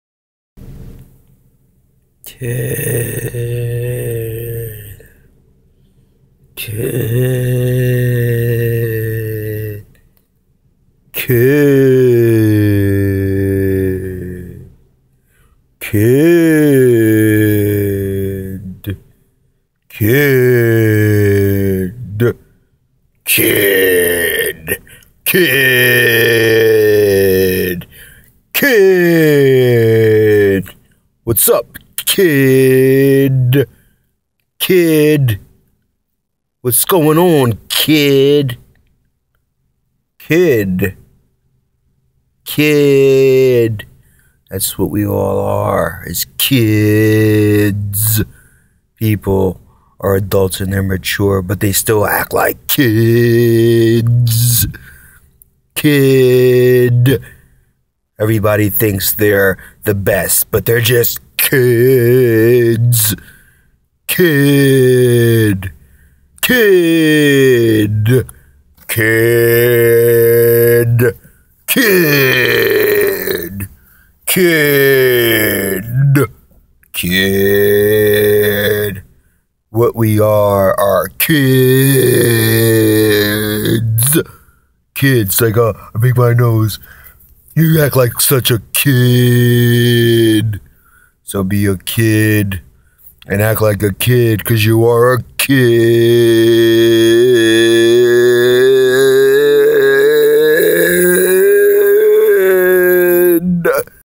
Kiiiiid
A man tells the world of how people are and how they truly act.